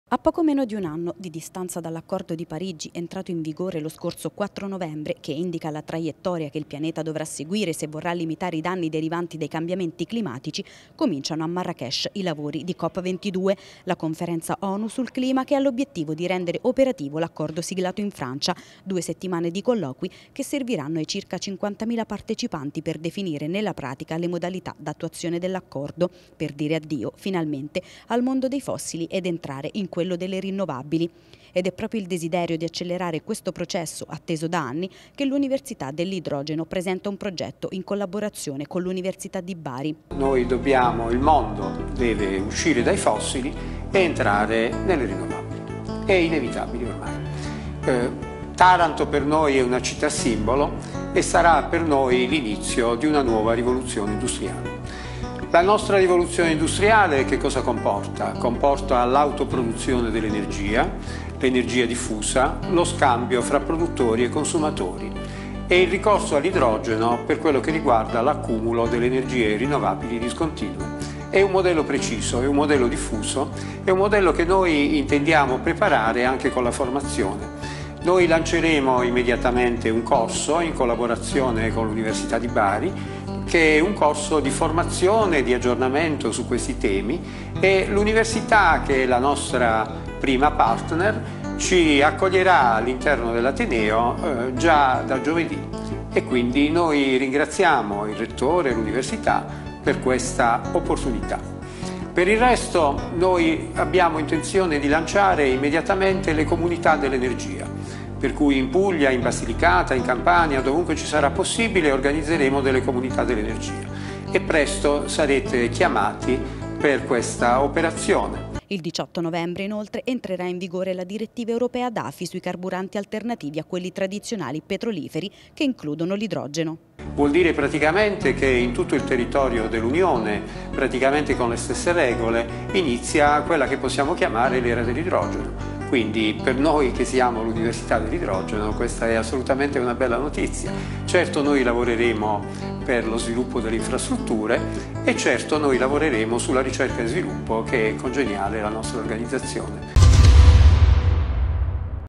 CLIMA, COP22: IL CAMBIAMENTO E' INIZIATO
A poco meno di un anno di distanza dall’accordo di Parigi, entrato in vigore lo scorso 4 novembre, che indica la “traiettoria” che il Pianeta dovrà seguire se vorrà limitare i danni derivanti dai cambiamenti climatici, cominciano a Marrakech i lavori di Cop 22.